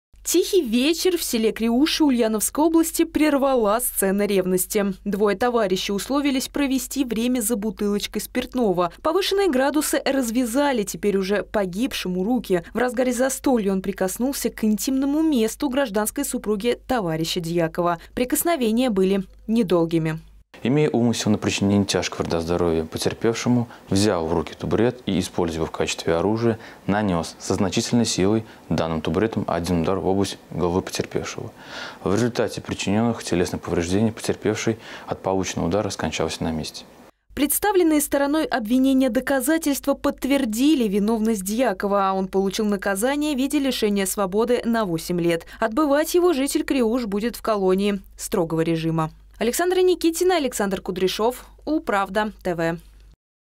Потрогал запретное. Жителя Криуш отправили за решетку за убийство
Начали за здравие, закончили за упокой. Двое мужчин из Ульяновской области решили скоротать время за горячительными напитками. В самом разгаре трапезы гость начал распускать руки и приставать к жене товарища, за что и поплатился жизнью.